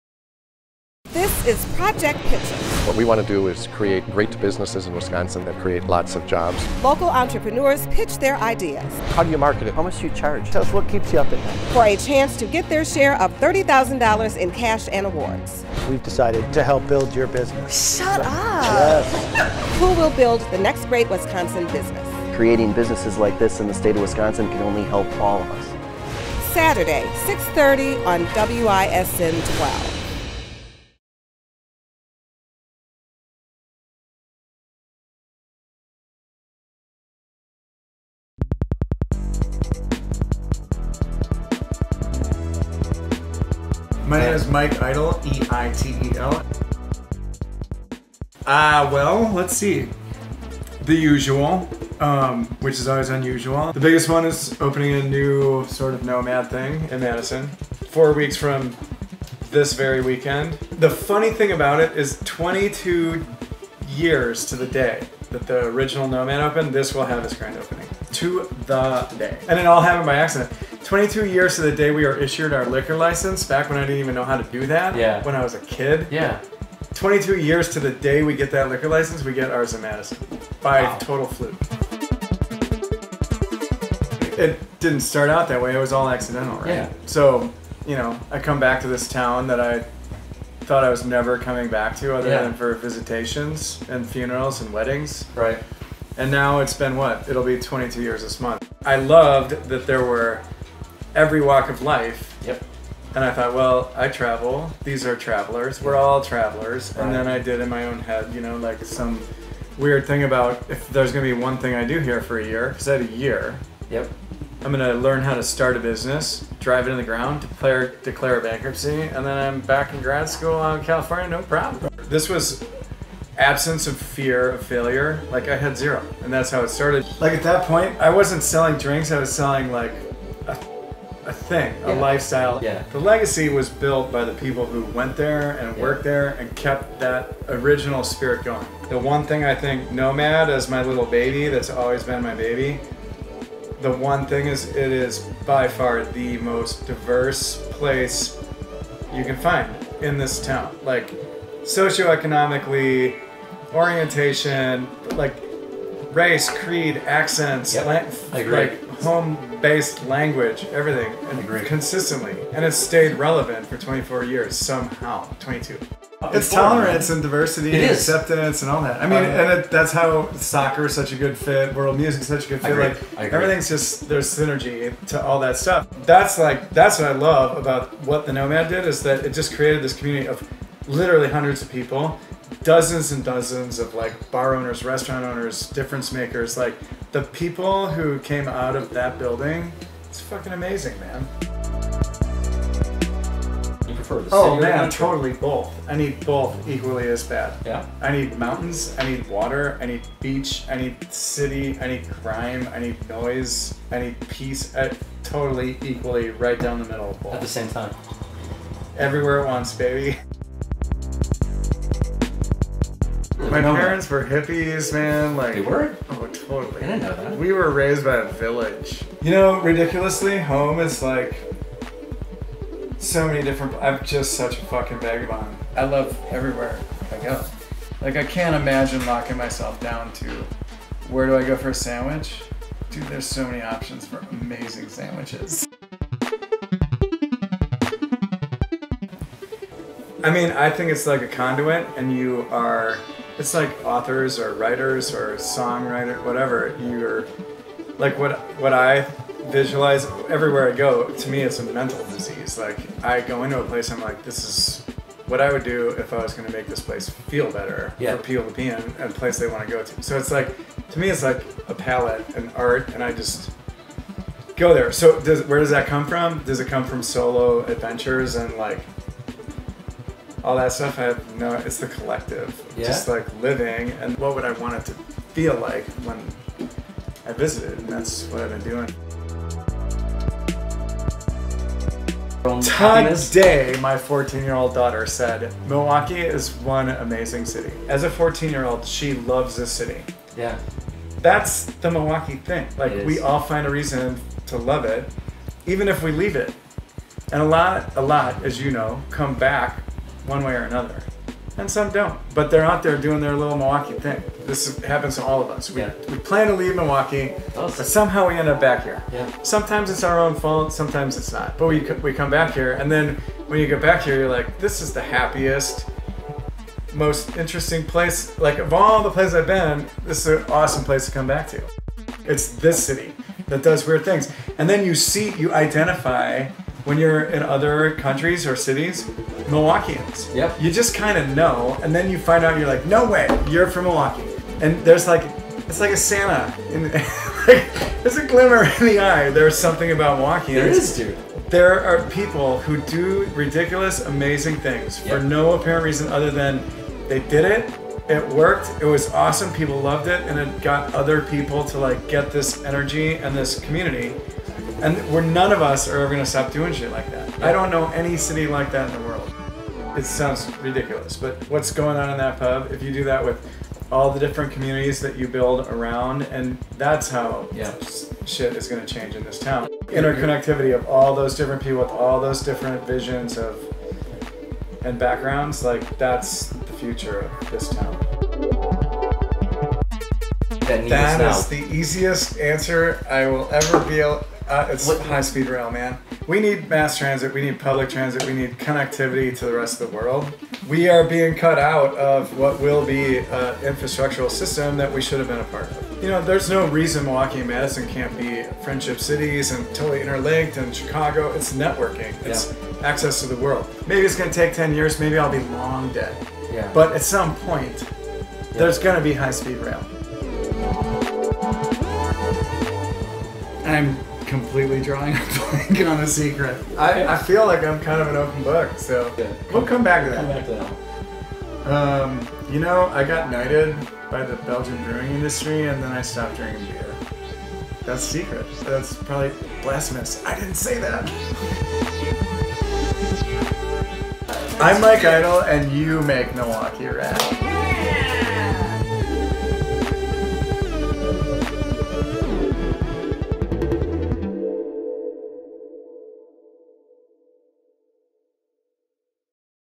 Nomad World Pub Entrepreneur Mike Eitel Makes Milwaukee Rad
This week's Make Milwaukee Rad, presented by Project Pitch It, features Mike Eitel who has been pioneering Milwaukee culture for the last 22 years. Since opening the Nomad World Pub on Brady St., he has directed and maintained his vision for a real space for all kinds of people.